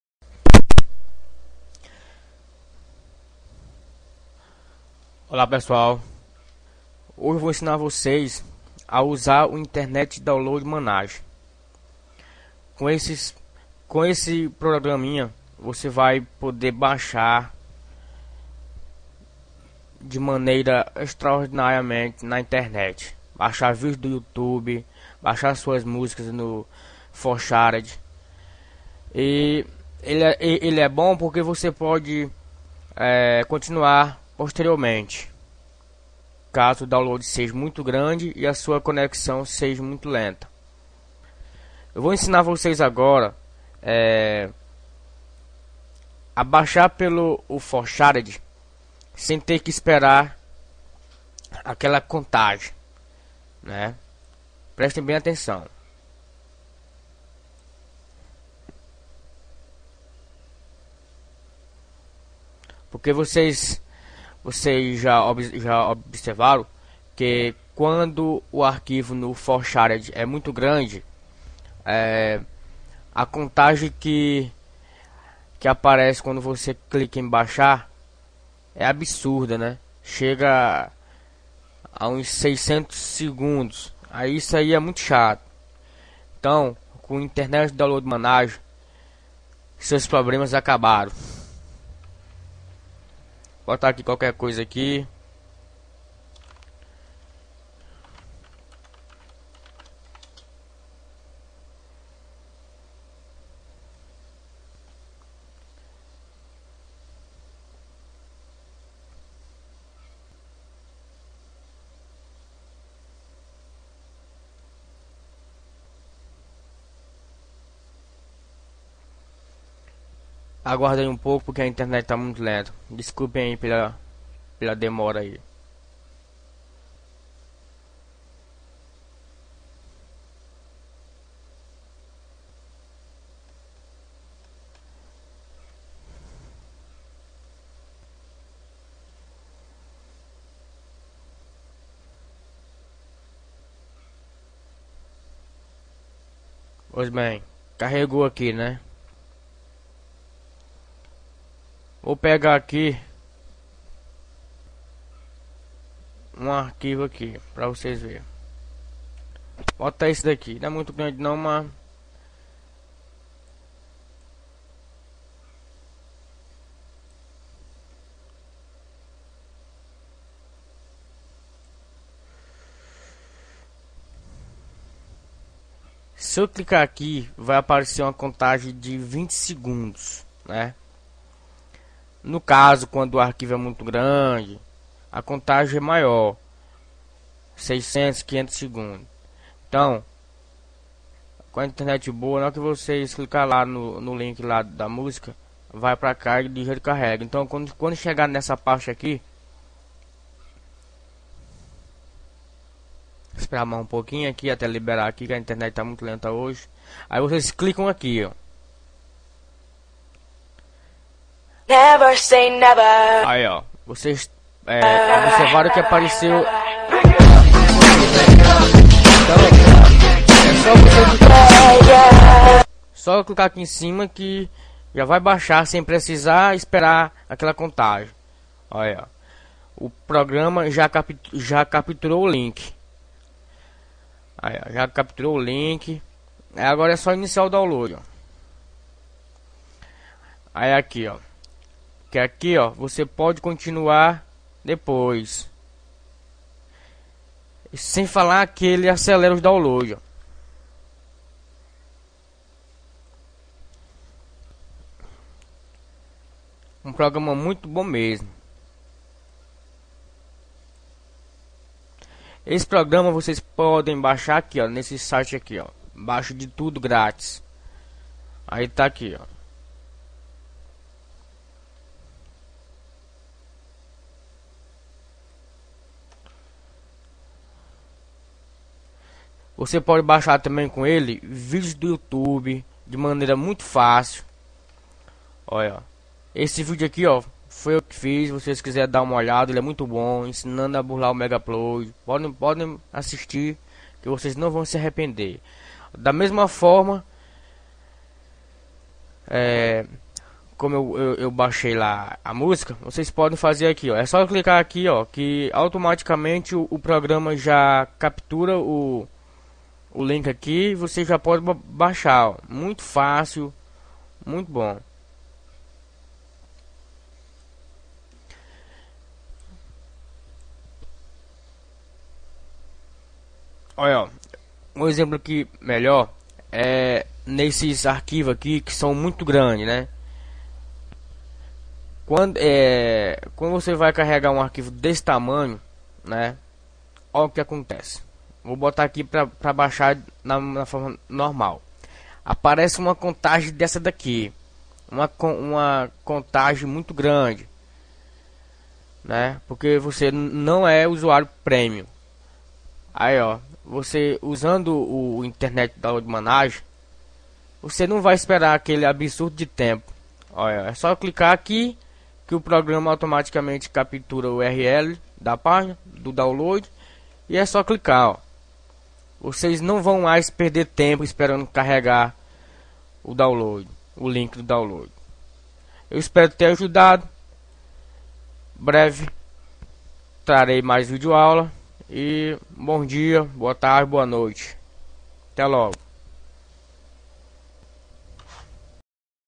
Internet Download Manager Como baixar videos, musicas e programas
Nunca mais espere aquela contagem do 4Shared chegar ao fim para que você possa fazer o Download. Assistindo a essa vídeo aula você irar baixar sem limites e de forma muito rápida sem ter que esperar até 600 Segundos para poder baixar seus arquivos Sem falar que com esse programa você poderá baixar vídeo de Qualquer Site. Assista até o fim e Depois deixe seu comentário!